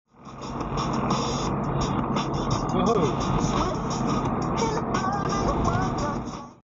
School bus Getting Air Time
School bus driver cuts a corner and gets a wheel off the ground.
Junction of Comber Road and Old Dundonald Road, Dundonald, County Down.